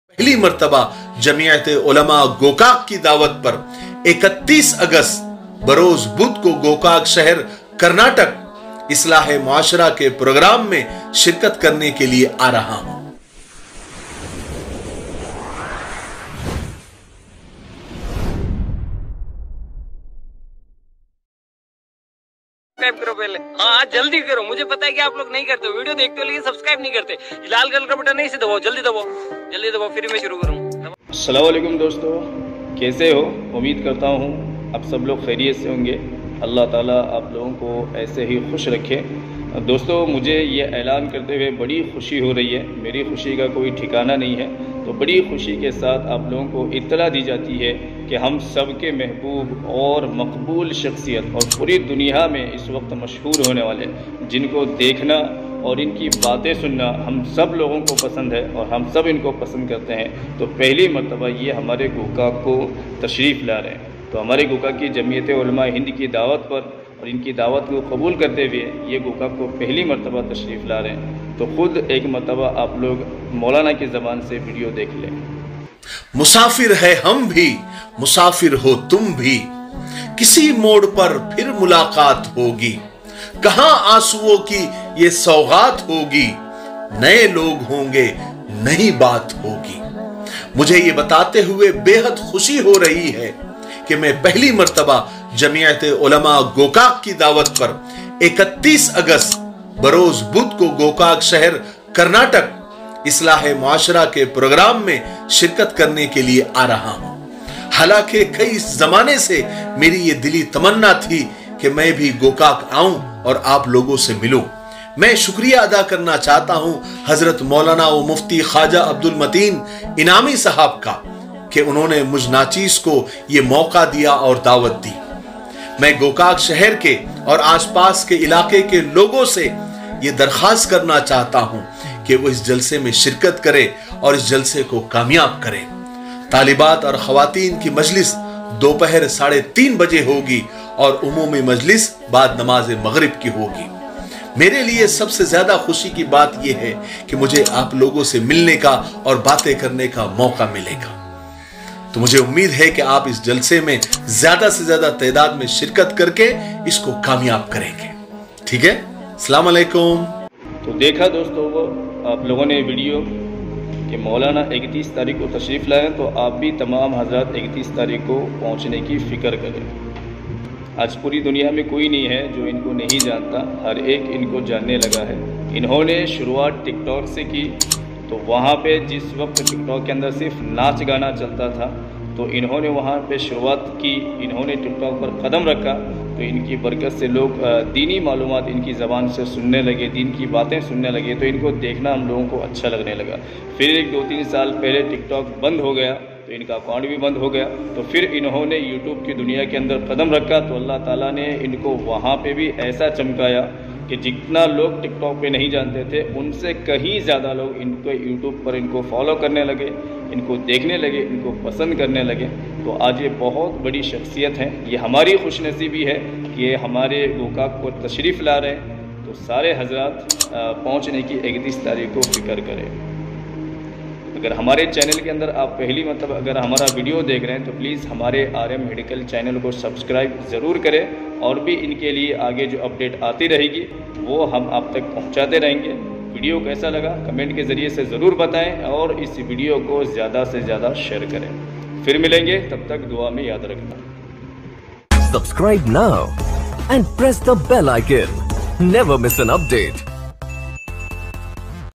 Breaking news || Gokak|| Molana Abdur Rashid Miftahi Puna || caming Soon || 31 08 2022
ikbar zarur dekhe frends aor aage sher zarur kare

YouTube k Bade ⭐⭐⭐⭐
 Jane mane hasti Molana Abdur Rashid Miftahi Gokak  tashrif larahe Hai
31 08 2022